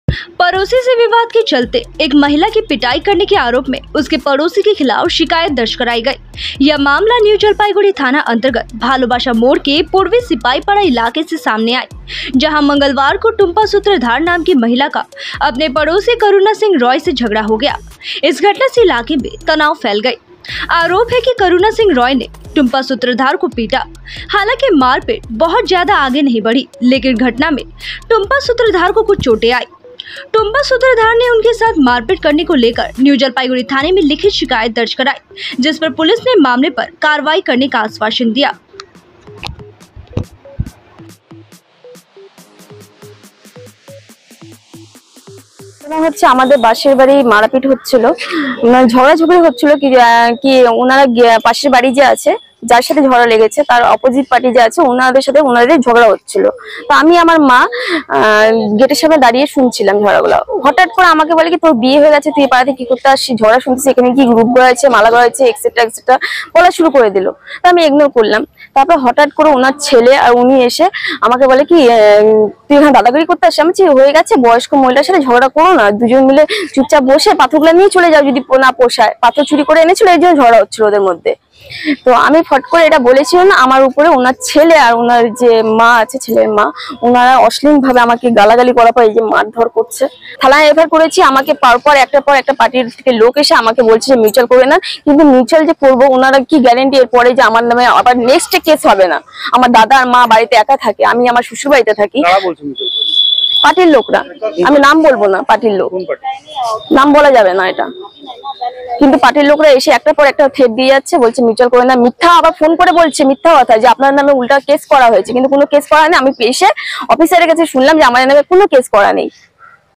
Fight broke between two neighbours; Complaint lodged in police station
Siliguri : Fight broke between two neighbours;  Complaint lodged in the police station regarding the incident